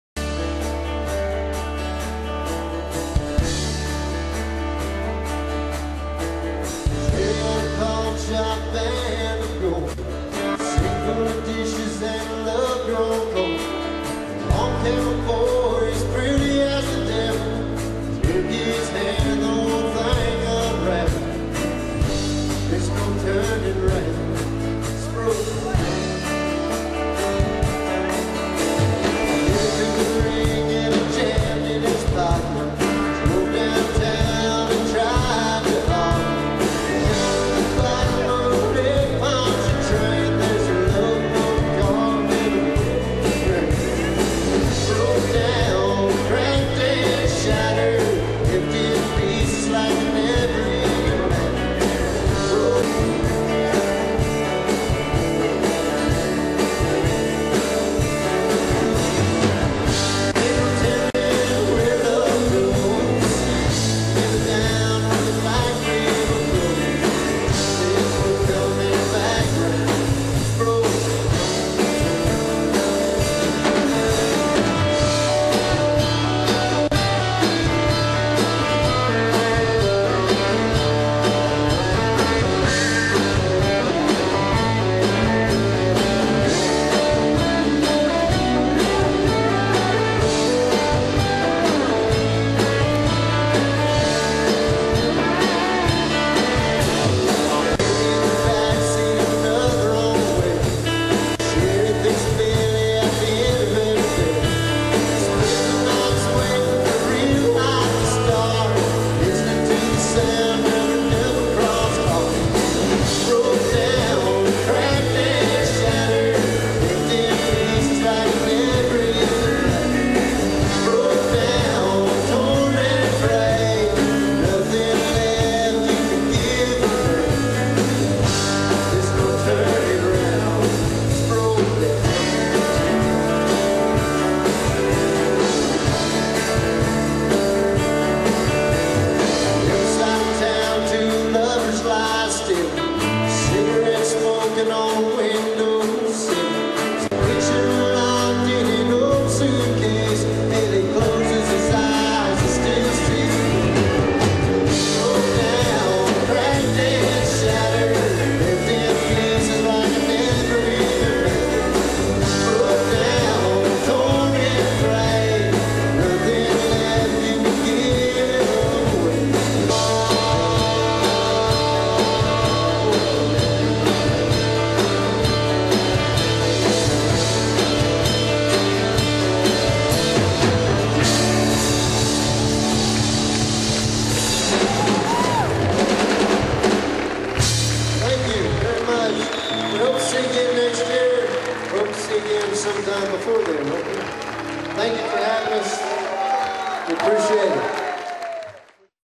Rodney Hayden - Equiblues France 2004
Extrait video du concert de Rodney Hayden le 13 août 2004 à Equiblues, France.